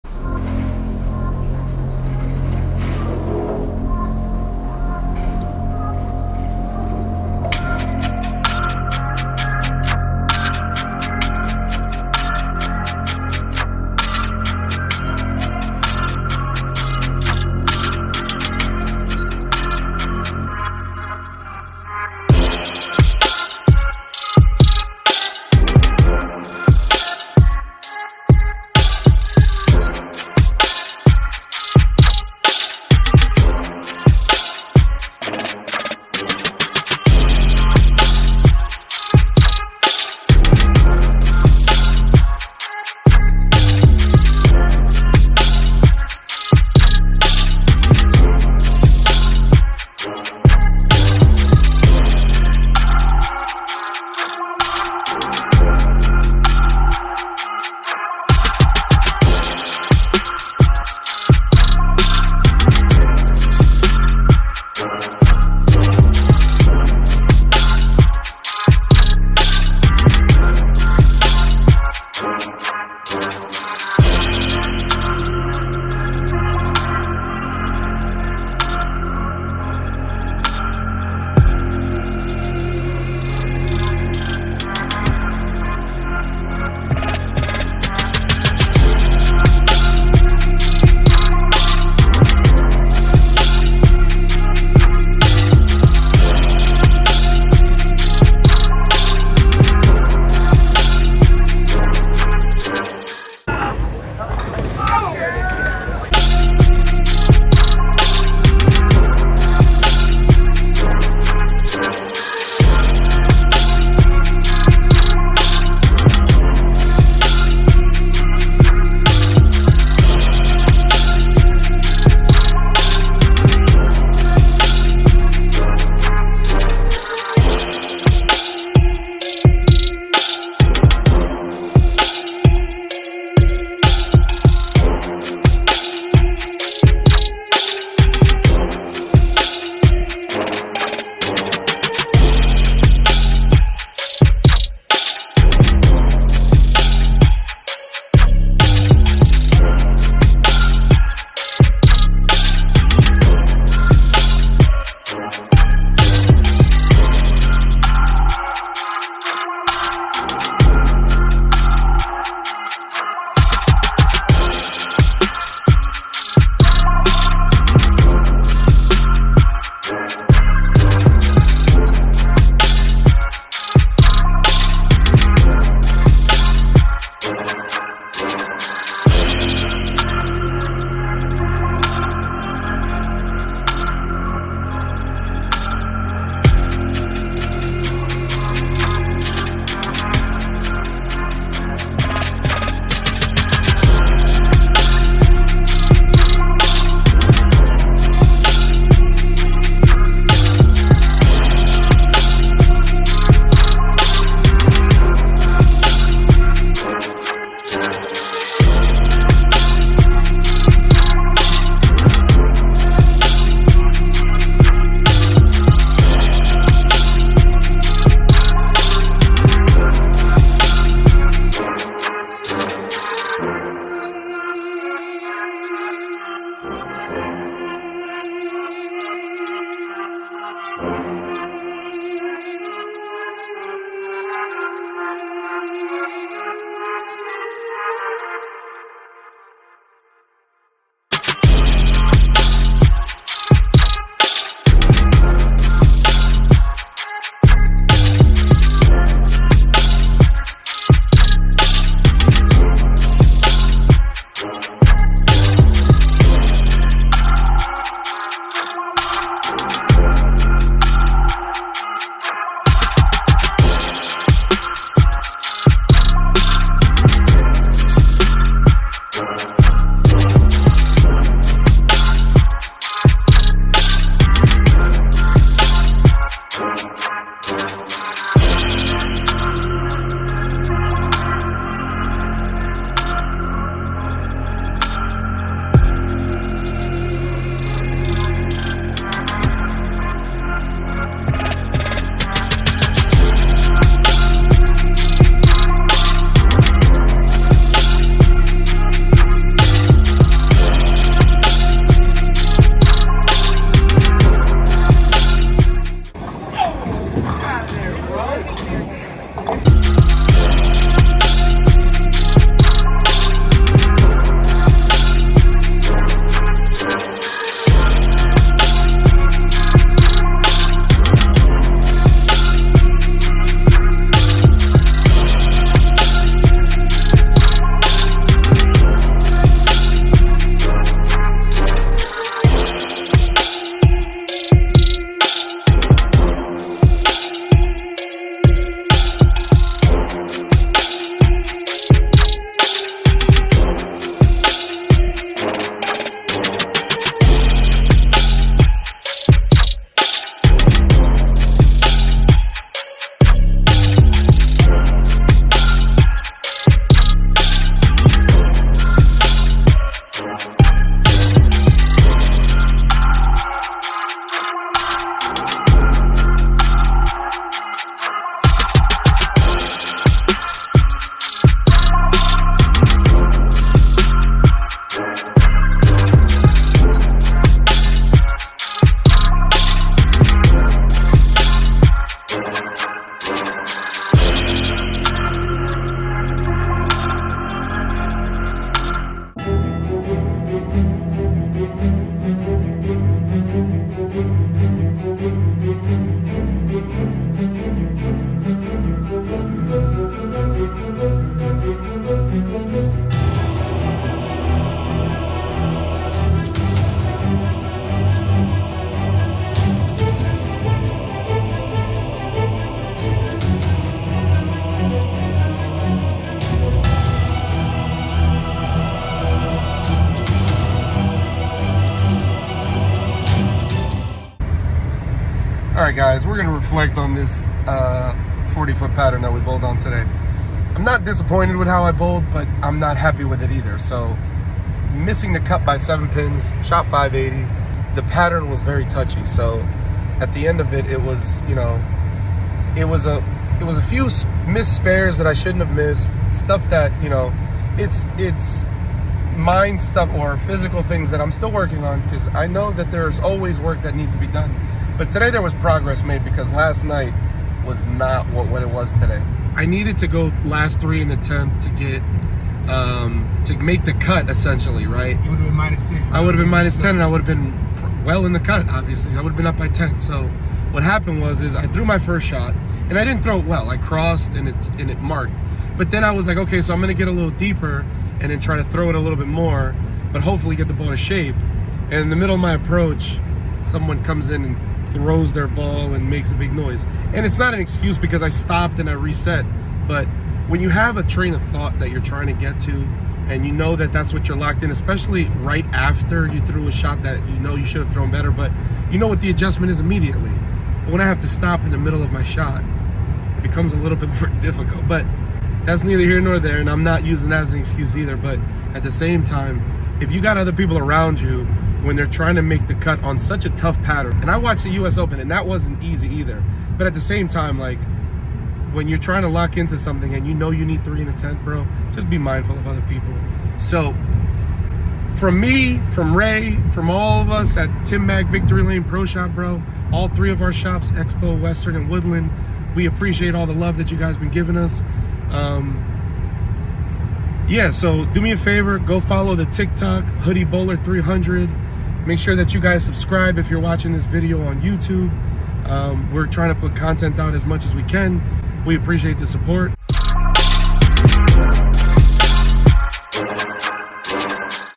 Did I make the cut? Or fall short? Indiana USBC Masters
Got a chance to bowl the Indiana usbc masters at championship lanes on Saturday at 8:30! Did I make the cut? or miss it? find out above!!!